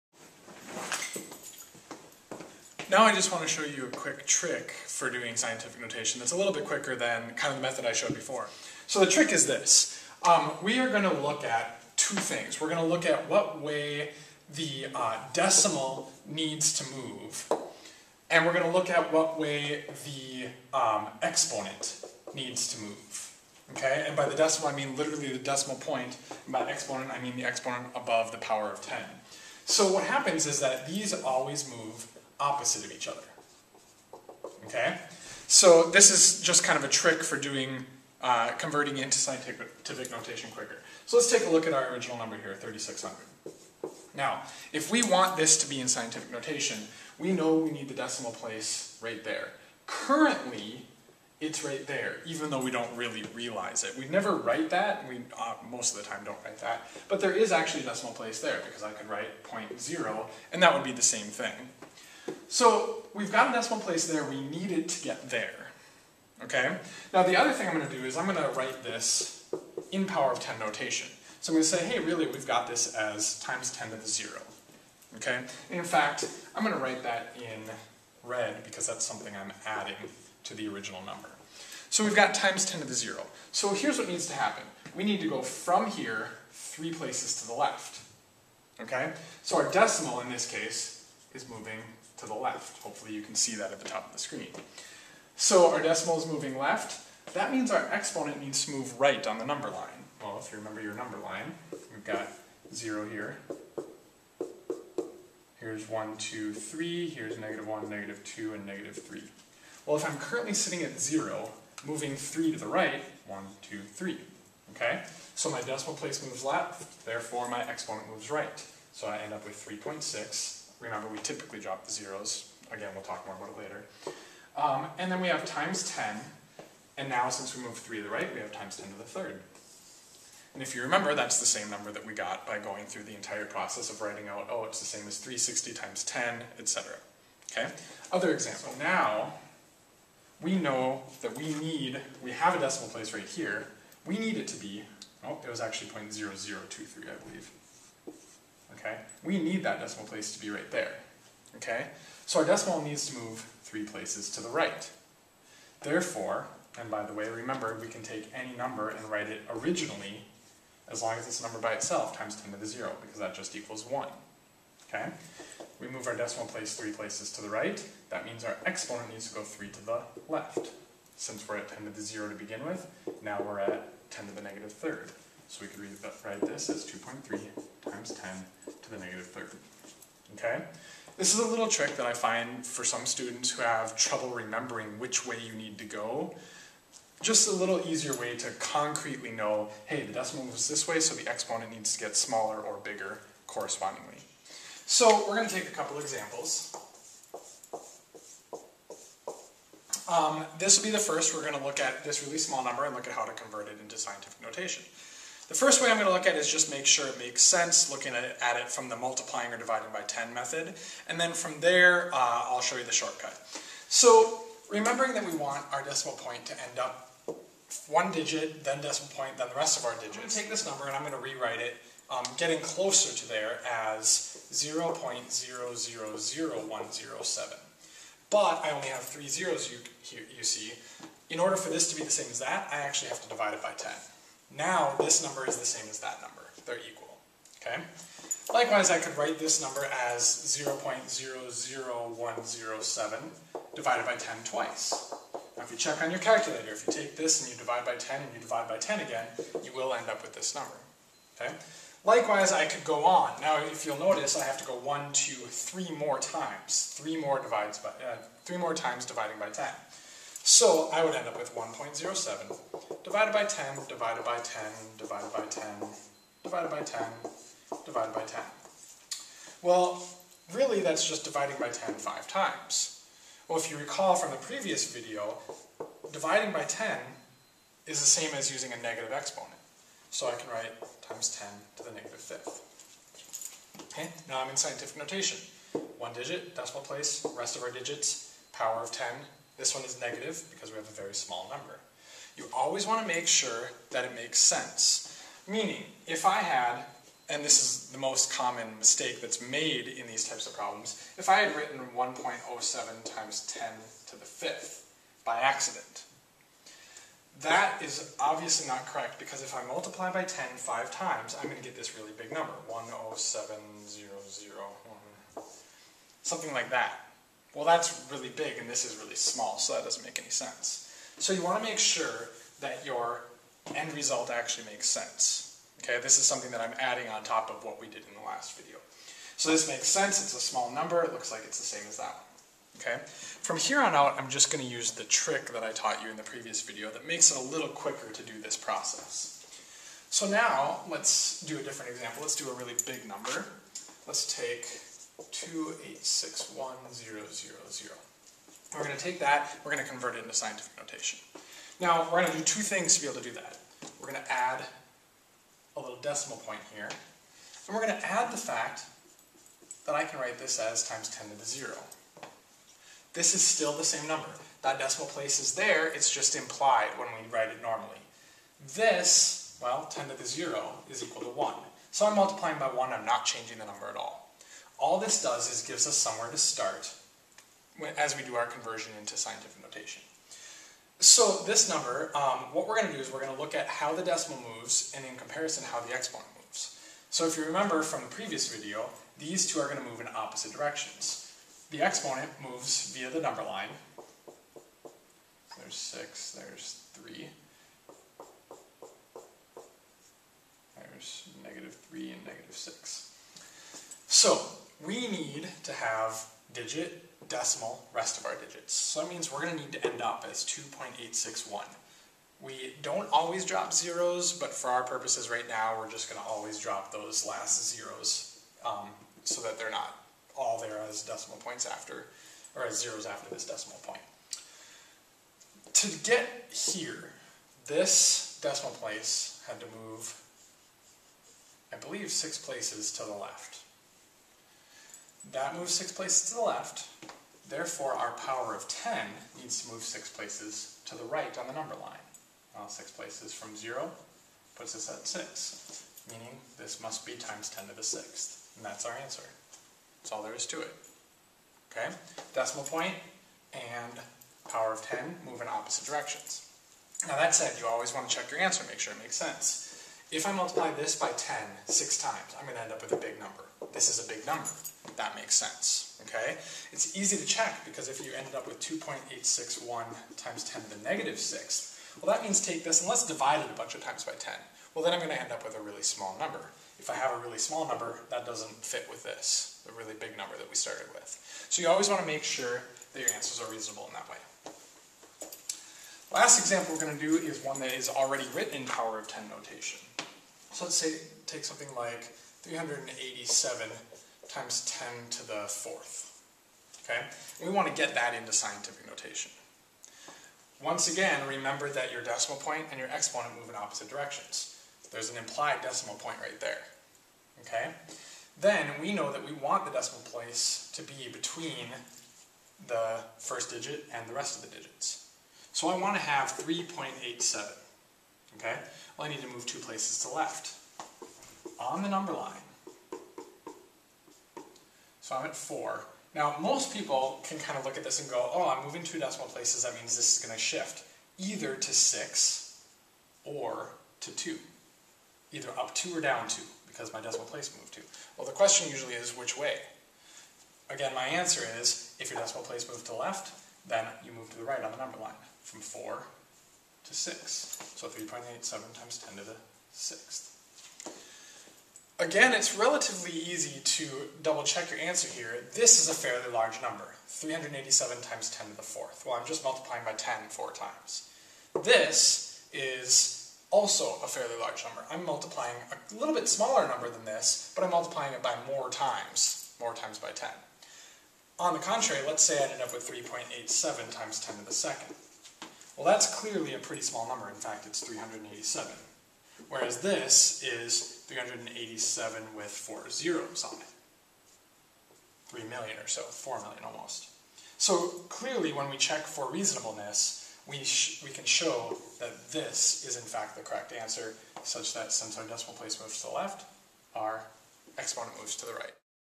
2 Scientific Notation Examples
This is the second video in a series of 8 (planned) videos to review scientific notation, scientific prefixes, and conversions for students in High School Physics. This video focuses on what Scientific notation actually means, with shortcuts and more examples to come in the next video.